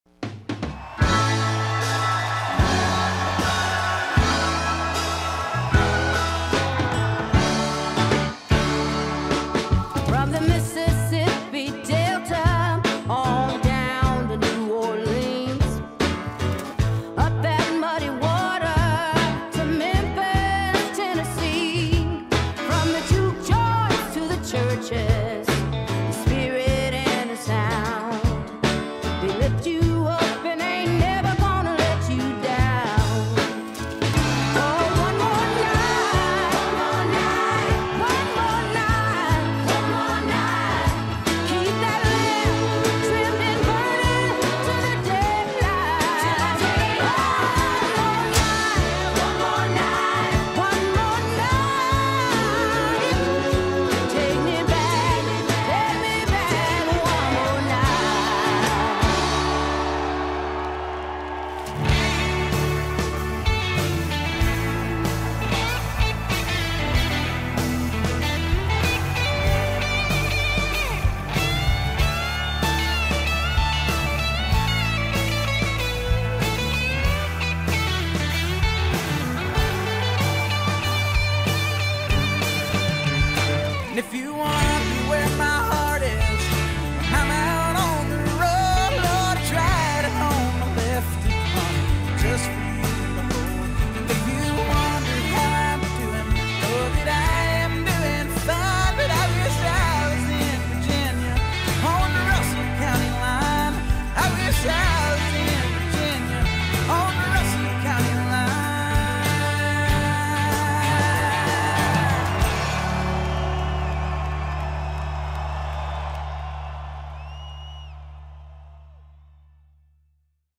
Bristol Rhythm & Roots Reunion - 2022 Recap
Established in 2001, the annual Bristol Rhythm & Roots Reunion music festival celebrates Bristol, Tennessee-Virginia’s music heritage as the birthplace of country music. Showcasing the very best in Appalachian roots music and beyond, the event honors the legacy of the 1927 Bristol Sessions and their far-reaching influence on the soundtrack of today.

Learn more and punch your ticket to the 2023 festival at BristolRhythm.com